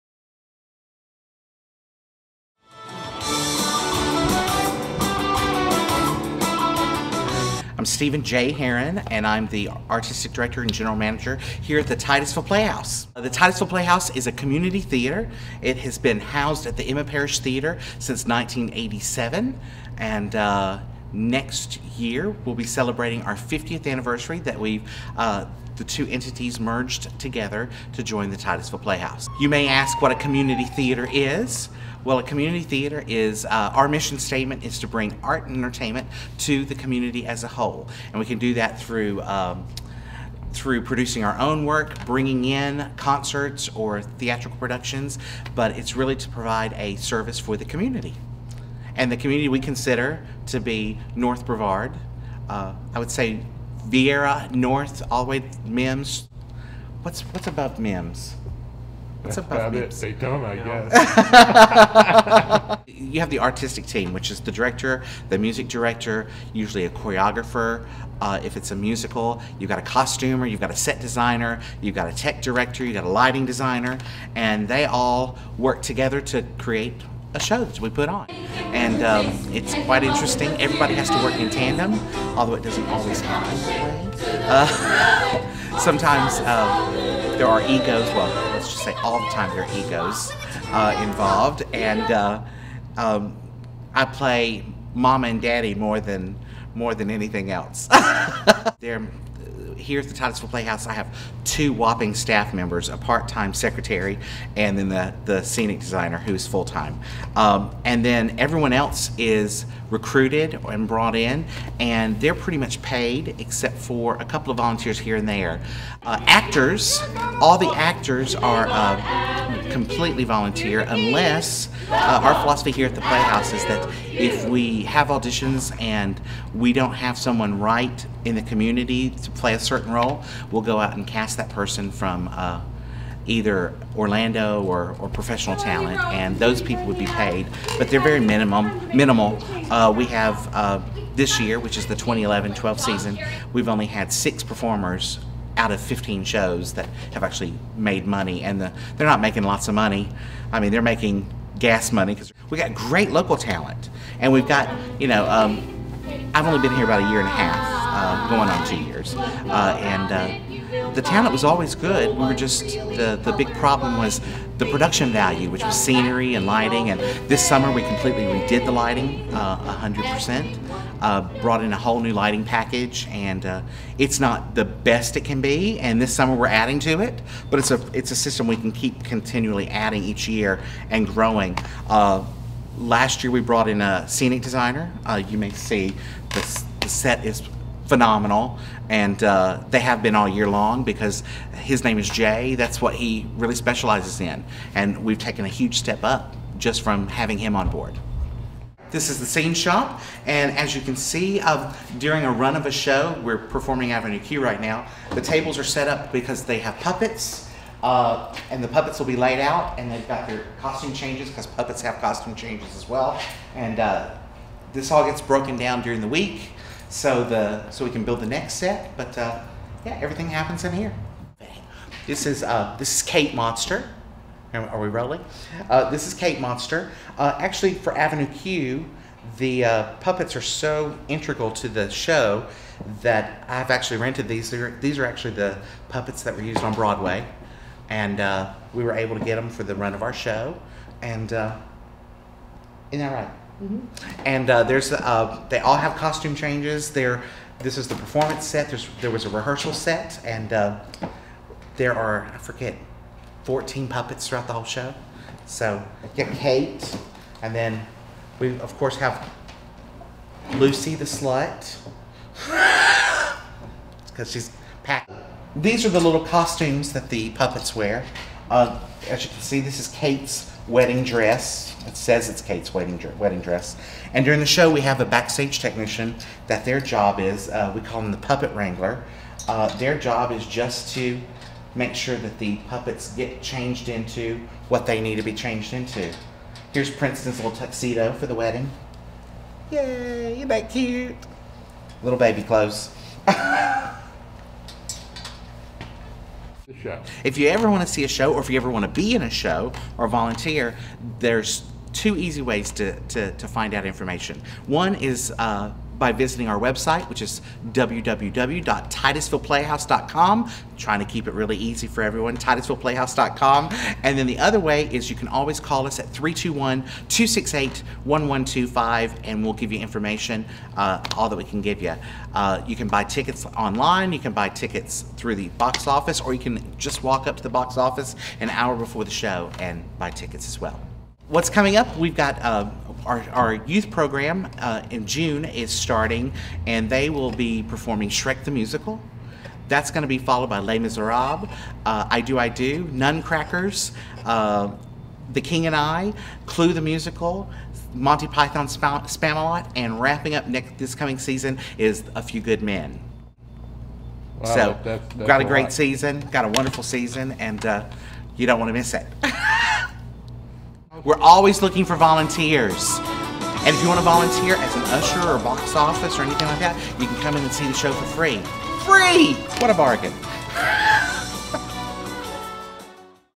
Titusville Playhouse Inc. Infomerical
Steve Heron, General Mgr. of the Titusville Playhouse is interviewed.